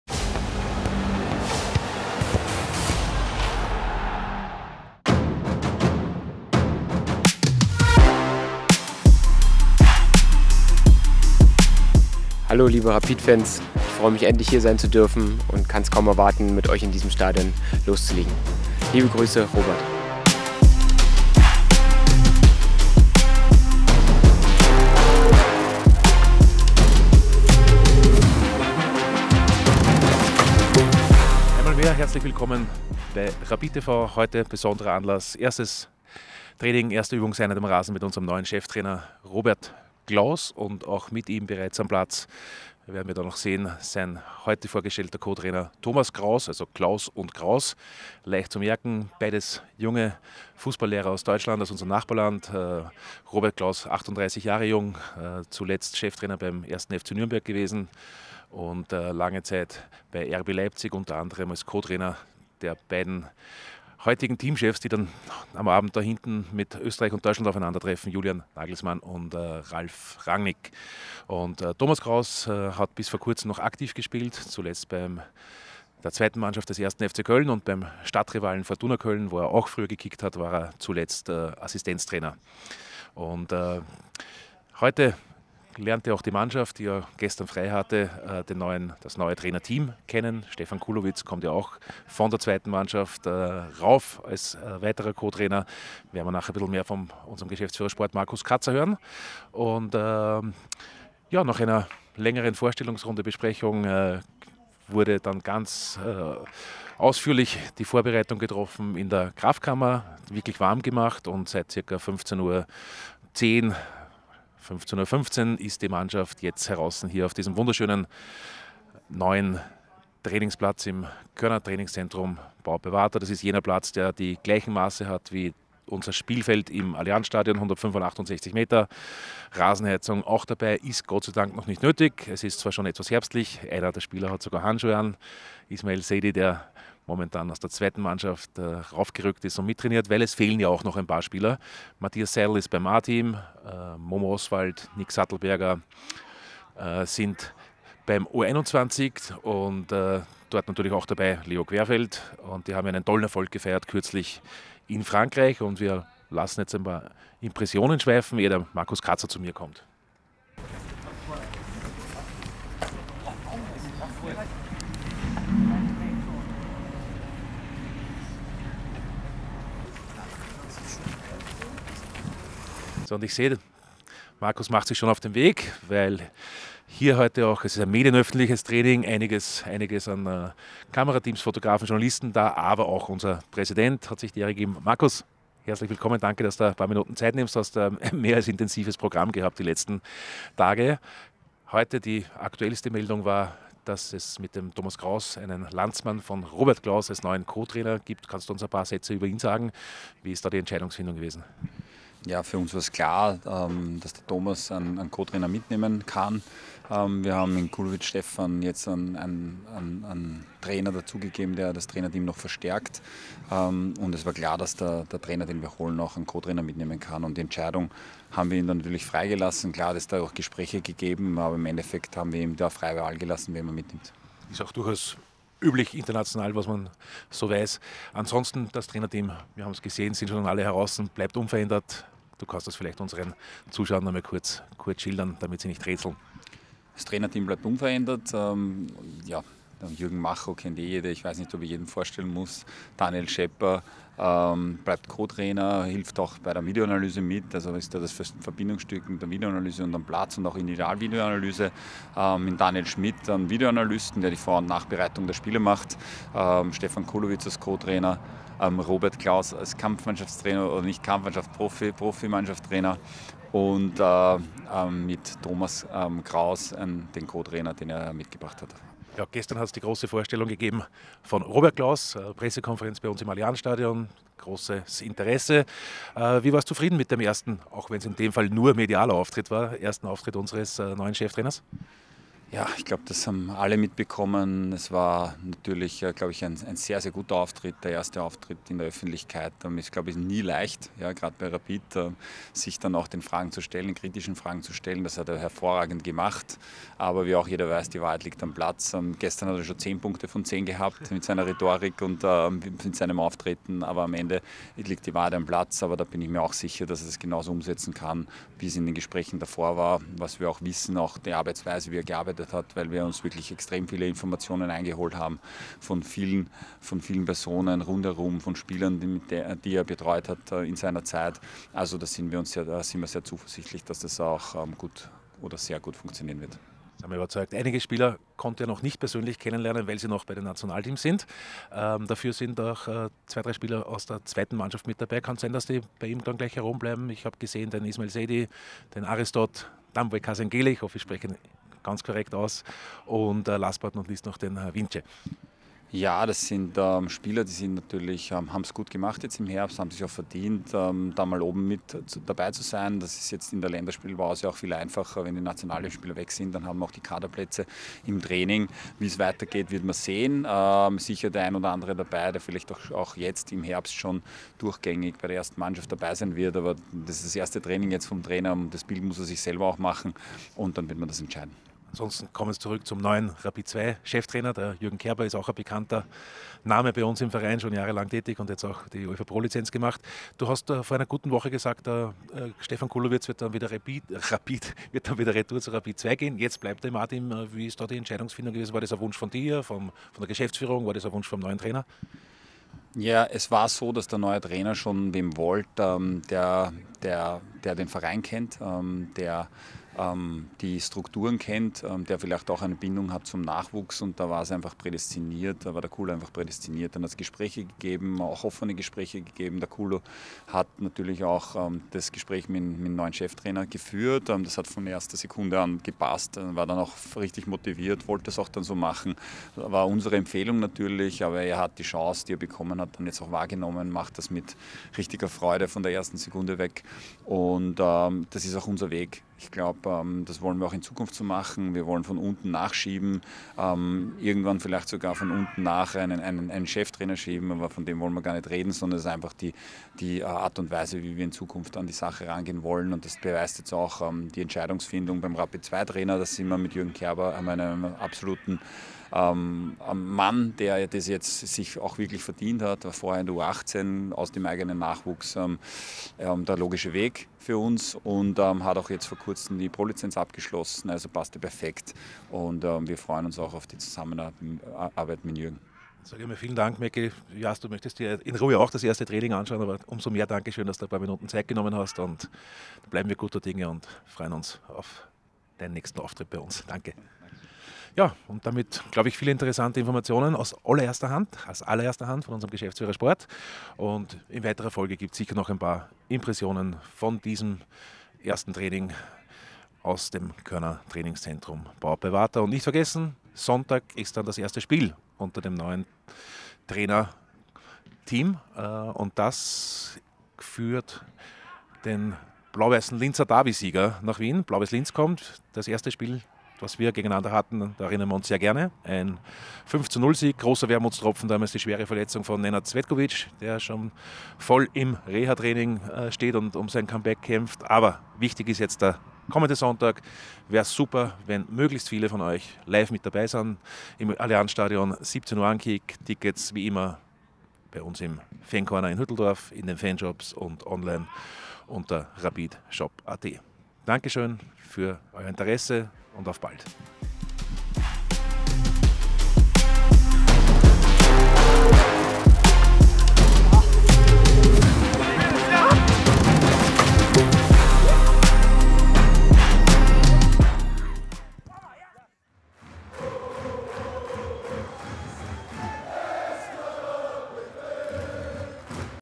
Trainingsstart unter Robert Klauss
Unser neuer Cheftrainer Robert Klauß und seine Co-Trainer Thomas Kraus und Stefan Kulovits beim ersten Training mit der Mannschaft.
Geschäftsführer Sport Markus Katzer mit einem kurzen Interview.